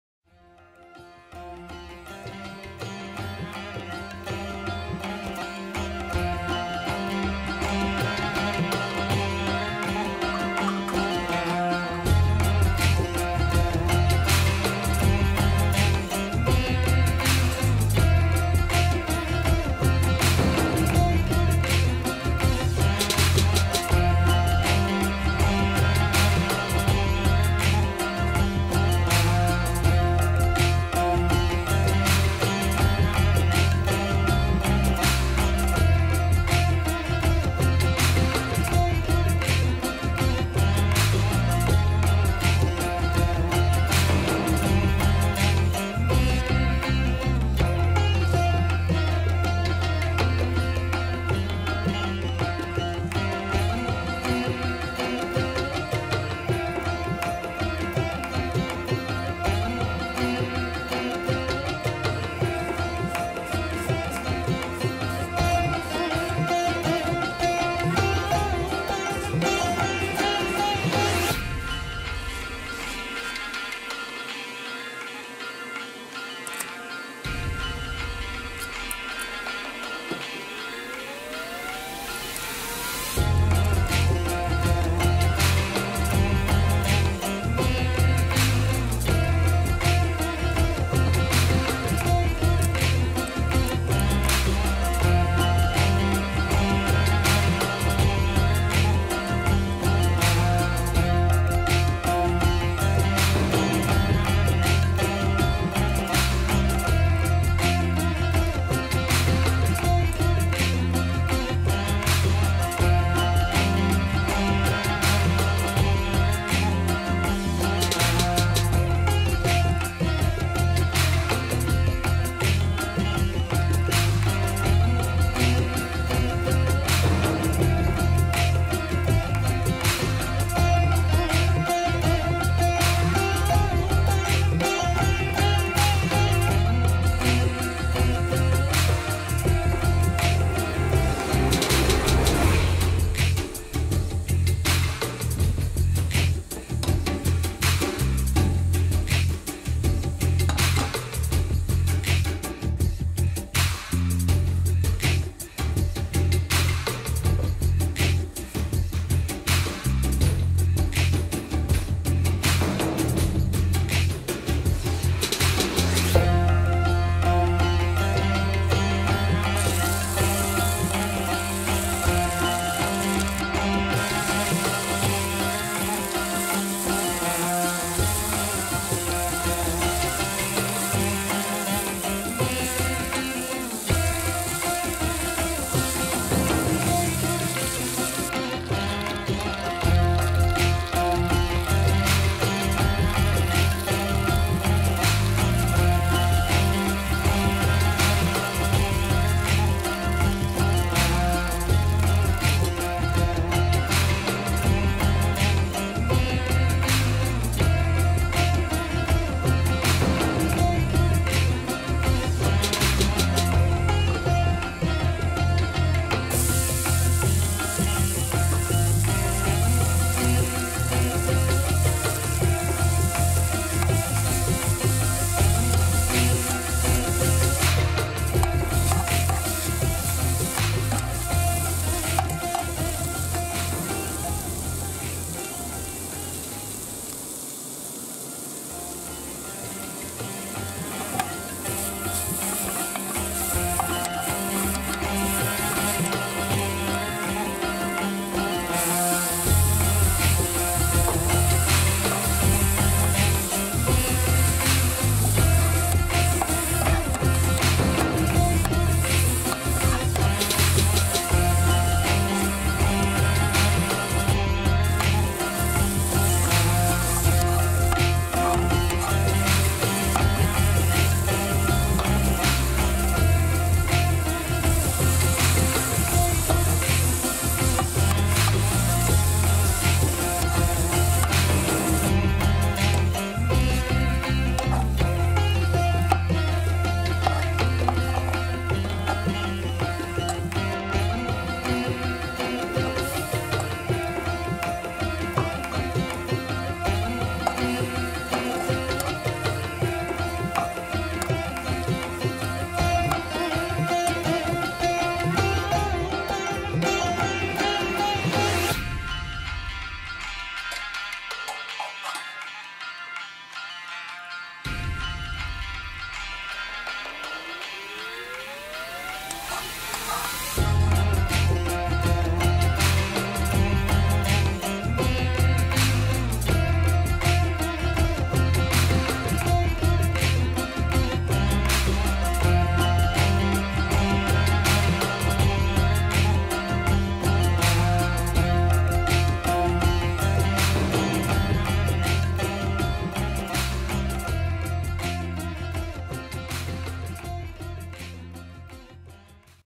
Spicy Fried Pomefret
A great center piece for the dinner table, not to mention absolutely delicious! This is a very tasty fish and this is the best way to serve it. A very easy to make recipe. I hope you enjoy it as much as we do!